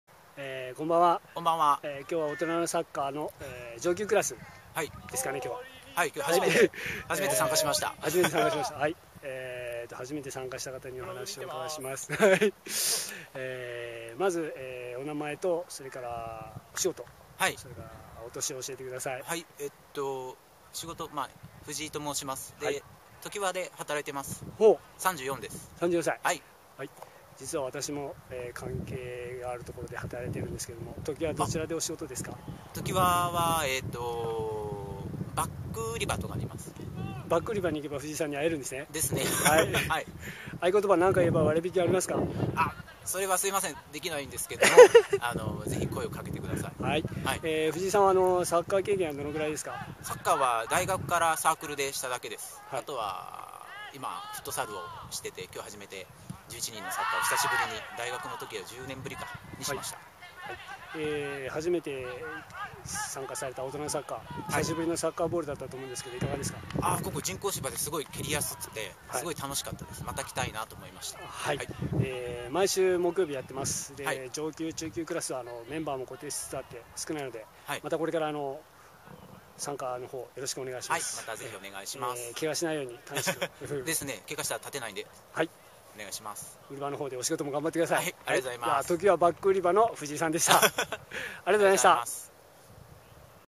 2010/06/03大分トリニータ 大人のサッカー上級クラス 参加者その１
今週の「大人のサッカー」、今回初めて参加！の方。
誰かに似てるなぁ・・・そう！松井大輔だ！似てる！真面目な受け答えがサスガに
普段のお仕事を表している感じでした、また来てください。